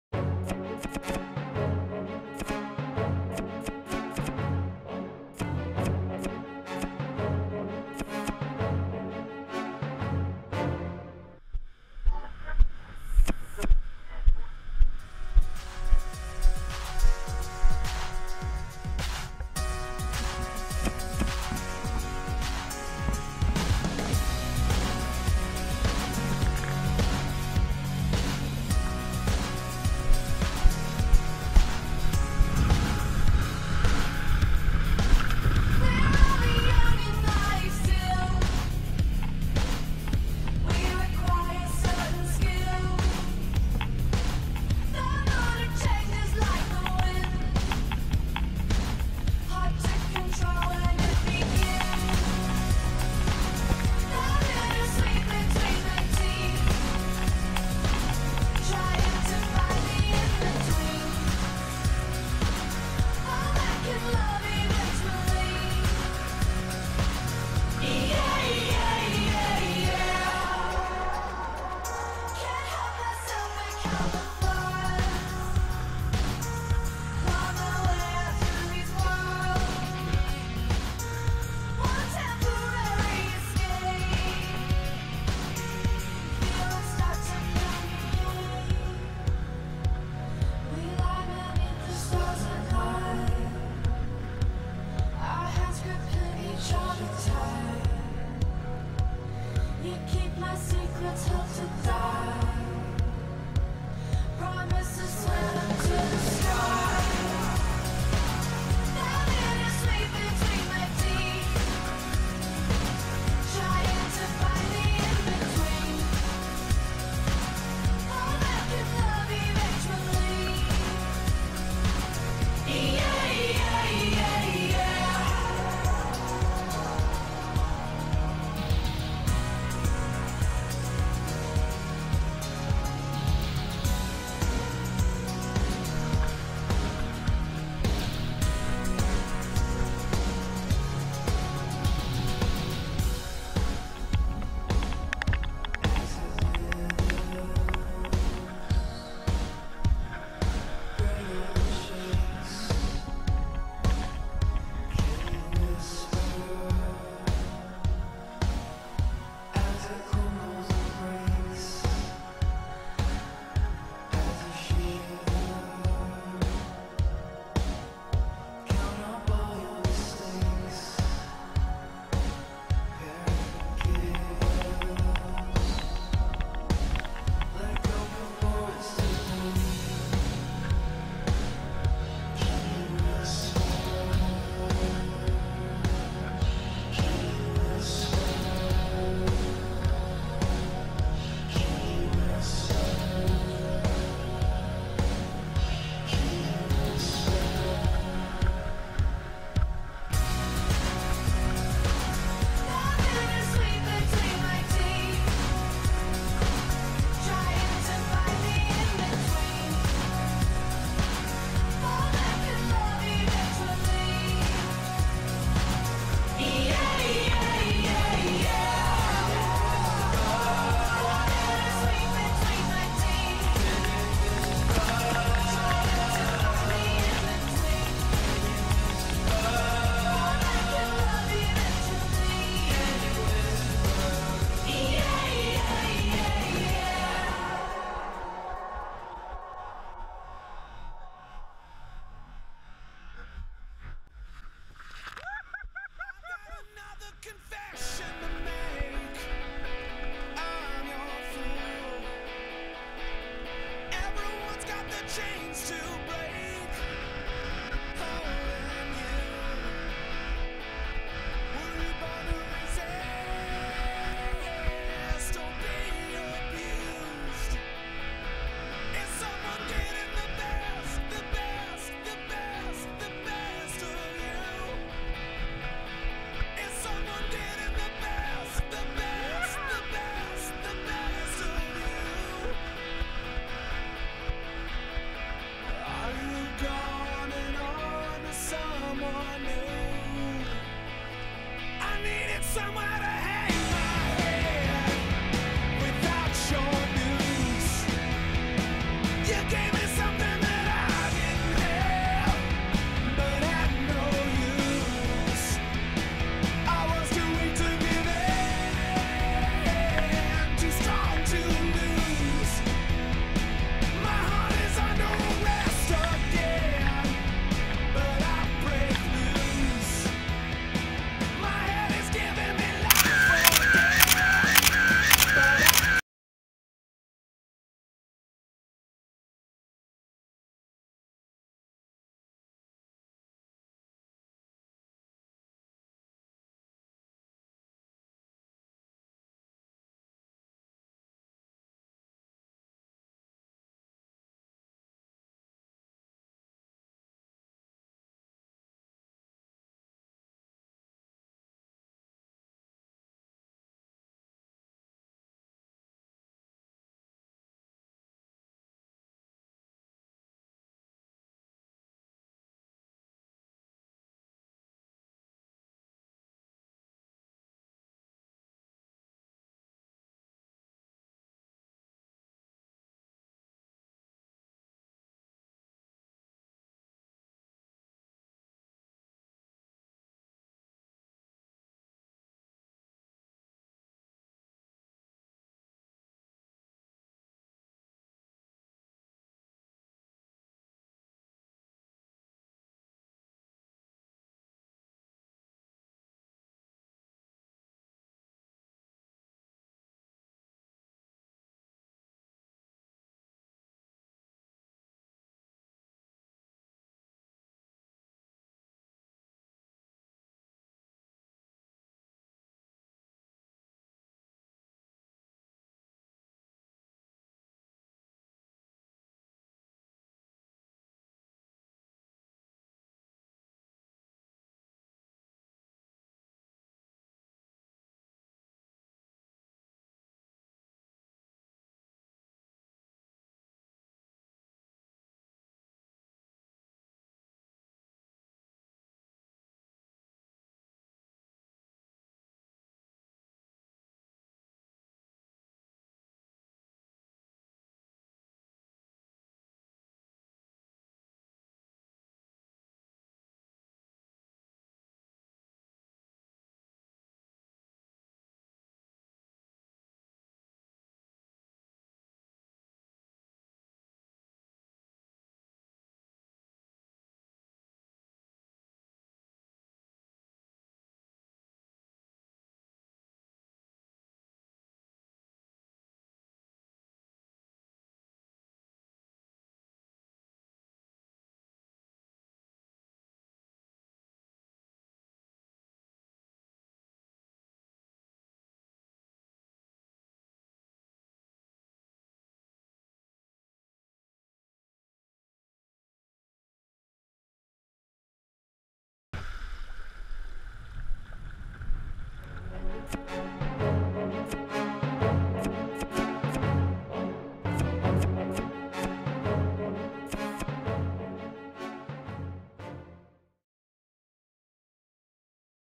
Surfsession 27.4.2013 St. Andrä am Zicksee
Erste surfsession in diesem Jahr
Location Zicksee Burgenland
Starboard Kode 80 Liter
Segel Naish Session 4,7qm